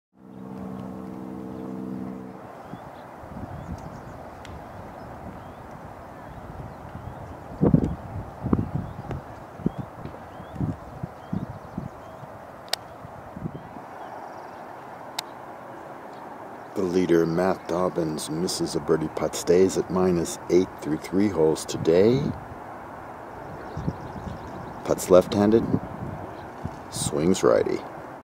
PGA National at Bayonet is The Major for non-touring golf professionals. 3rd hole round 3.
Some 8000 professional golfers attempted to qualify.  Under 300 made it, and last night the cut came for the surviving 70.  Matt Dobyns had an overnight 3 shot lead, but it has evaporated.  Kelly Mitchum, an assistant pro at Pinehurst is 3 under through 8 today and even with Dobyns at minus 7, who is plus one today.  3 time PGA National Champion Mike Small lurks a single shot off the pace.  Spectacular weather is gracing the Monterey Peninsula, but galleries are small for the most important of all Tournaments for Club Pros, College Coaches and PGA non-touring players.  Double bogie on #9 for Mitchum, so Dobyns alone at top heading into the turn of round 3 at Bayonet